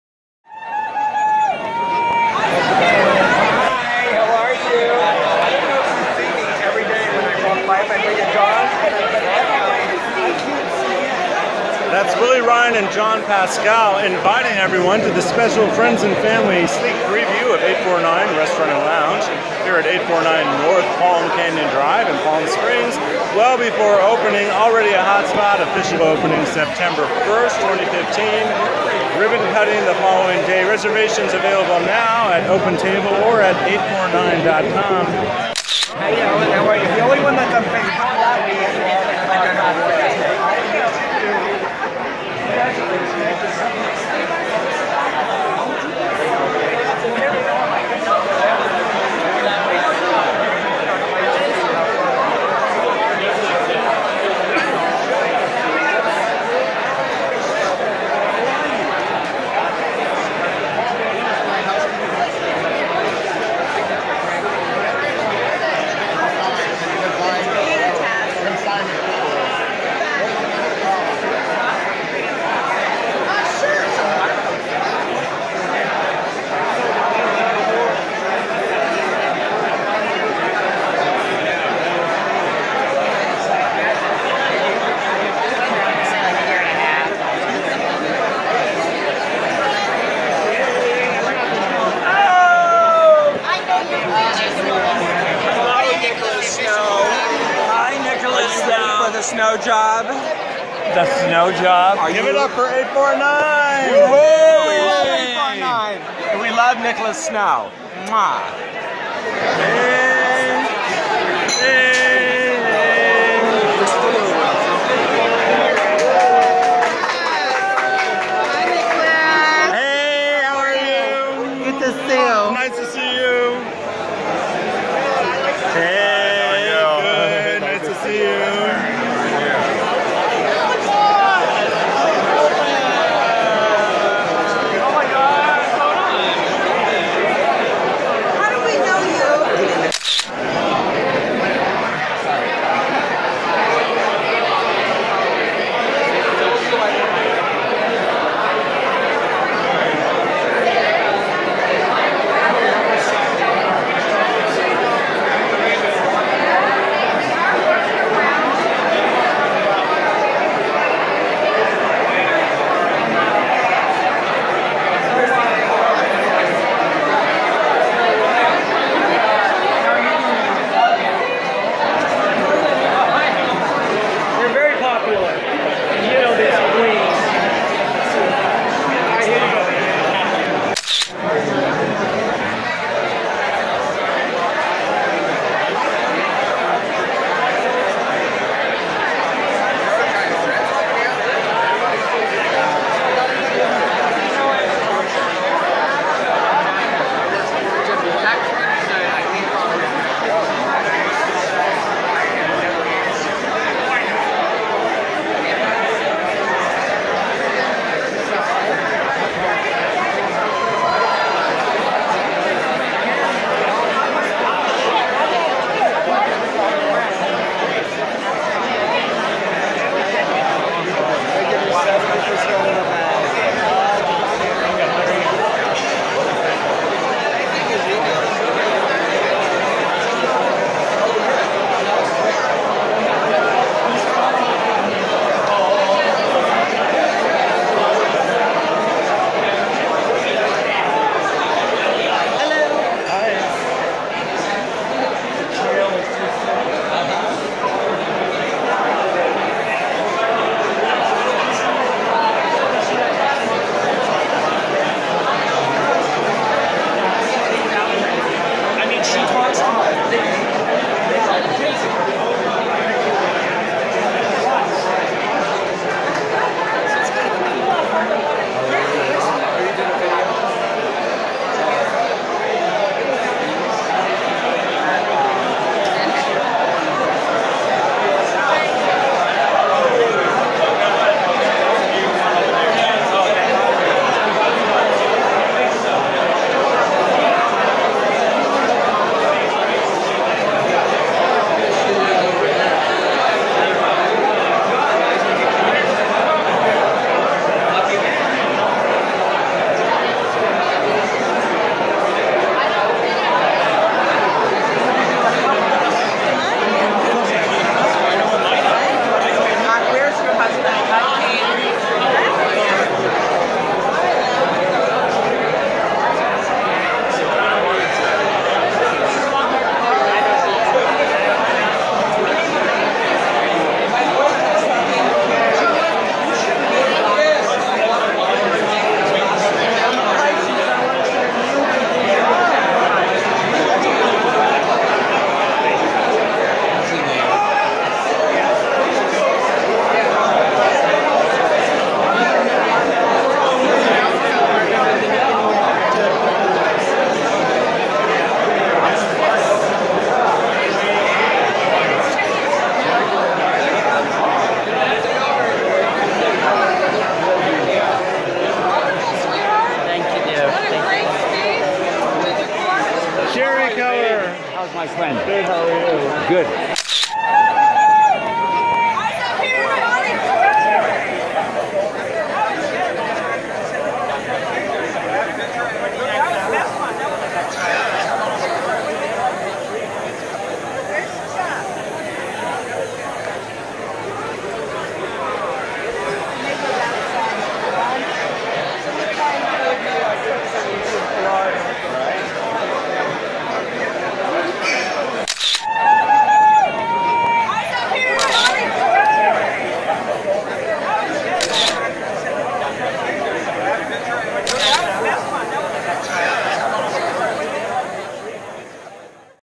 Eight4Nine Restaurant & Lounge Palm Springs: Friends & Family Sneak Peek!
On Saturday, August 29th, days before their September 1st, 2015 opening day, Willie Rhine & John Paschal proudly hosted friends and family for cocktails and Hors D'oeuvres, opening the doors of Eight4Nine, Restaurant & Lounge for the first time.  Nicholas Snow was on hand to capture all of the excitement.